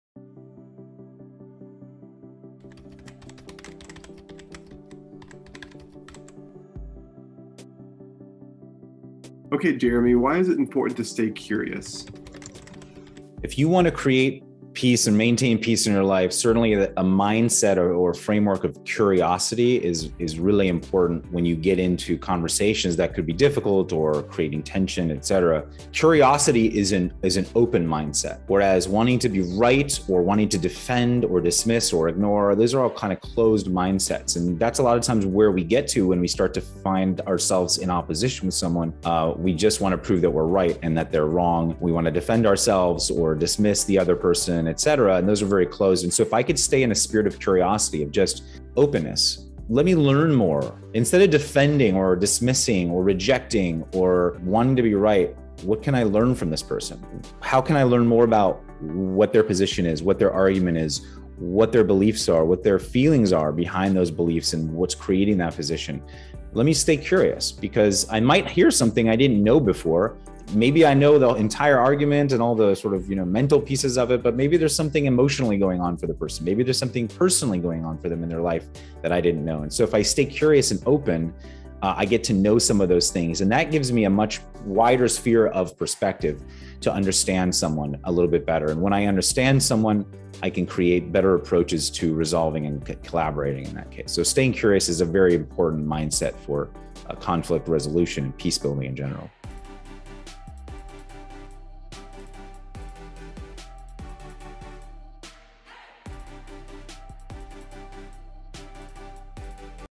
To Manage Conflict, Learn to Stay Curious by Dr. Jeremy Pollack, Workplace Conflict Specialist
Weekly Tips to Bring More Peace Into Your Life
Tip #30: Stay Curious

Staying curious is a great way to mitigate potential conflicts in situations in your life. Curiosity is an open mindset where you can understand the other party’s perspective better.

Sometimes we want to defend ourselves and prove the other person wrong, but that is a very closed mindset. Attempting to learn what their positions, arguments, feelings, and beliefs are helps us understand the other party better.

By staying curious, we get to understand someone better, and when we understand someone, we can create better approaches to resolving conflict.